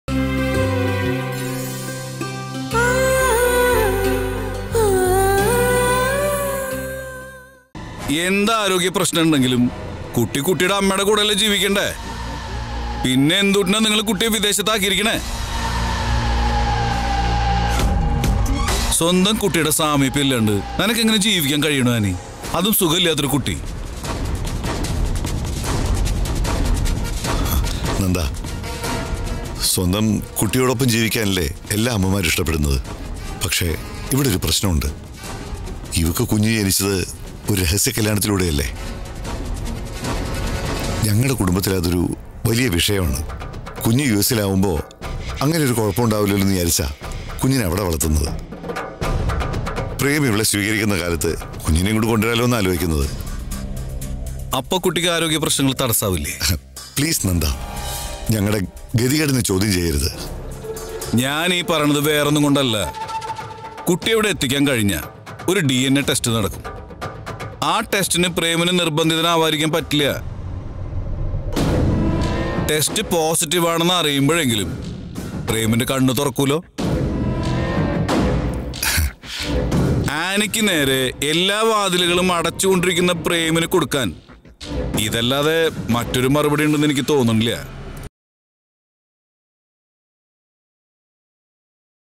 ആനിയുടെ കുട്ടിയെ വിദേശത്ത് നിന്ന് നാട്ടിലെത്തിക്കാൻ നന്ദകുമാർ നിർബന്ധിക്കുന്നു.
ആനിയുടെ കുട്ടിയെ വിദേശത്ത് നിന്ന് നാട്ടിലെത്തിക്കാൻ നന്ദകുമാർ നിർബന്ധിക്കുന്നു. അതോടെ ആനിക്ക് അടി പതറുന്നു